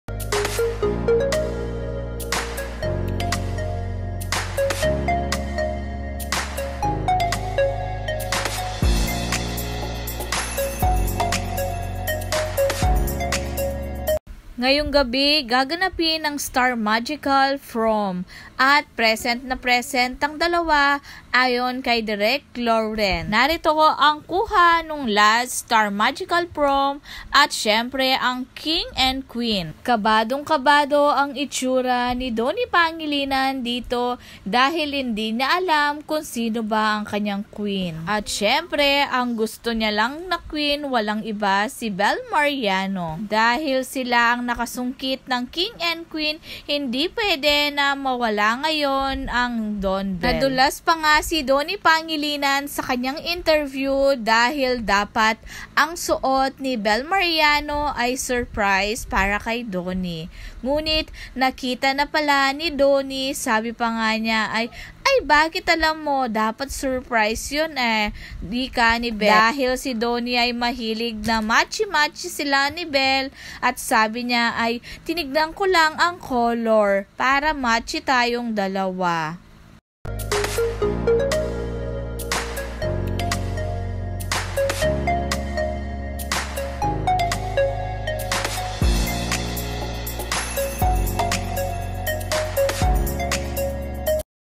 DONBELLE MULING NAG MATCHY SA STAR MAGICAL PROM
Tama nga sinabi ni donny p. Babagayan niya ang suot ni belle m. Grabe napaka kinang nilang tignan sakanilang matchy na damit sa star magical prom 2024